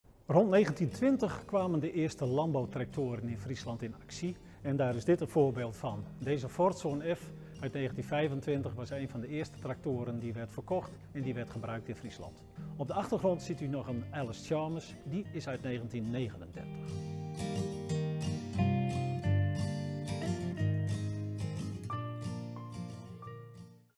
De Fordson F. in het Fries Landbouwmuseum
In Friesland namen rond 1920 verkopers als Boeke en Huidekoper (Case) en Hartelust (International) trekkers op in hun leveringspakket. Het eerst bekende trekker-optreden was in Oudwoude in 1920 toen een ‘International 8-16’ werd ingezet.
Het Ford model F, uit 1917 was de eerste trekker met een zelfdragende constructie waarbij motor, versnellingsbak en achteras één geheel vormden. Dit concept zien we tot op de dag van vandaag bij nagenoeg elke trekker nog terug.
De Ford F was vanaf 1922 in Friesland te koop. Het waren veelal autodealers die de trekker verkochten. De meeste trekkers werden eerst in de akkerbouw ingezet, vooral door loonwerkers. Aan het einde van de jaren 1930 kwamen er meer trekkermerken in Friesland beschikbaar. De rupstrekker deed zijn intrede in 1935. Daarnaast kwamen er eigen gebouwde ‘autotrekkers’. Na de Tweede Wereldoorlog ging het snel. Halverwege de jaren 1960 waren er evenveel trekkers als paarden in gebruik. Na de oorlog werd ook de (omgebouwde) ‘Jeep’ tijdelijk ingezet als trekker.